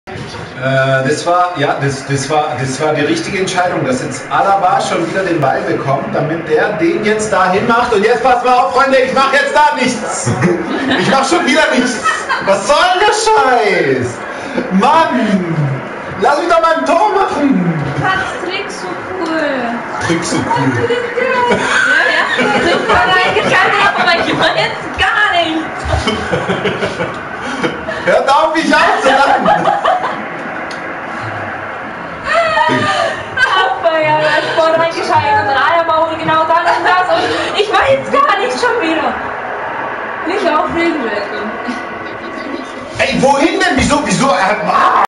Nicht aufregen Zeljko... (Highlight vom 02.11.2014)
Alaba am Ball und er geht und macht und dann macht er... garnix... :)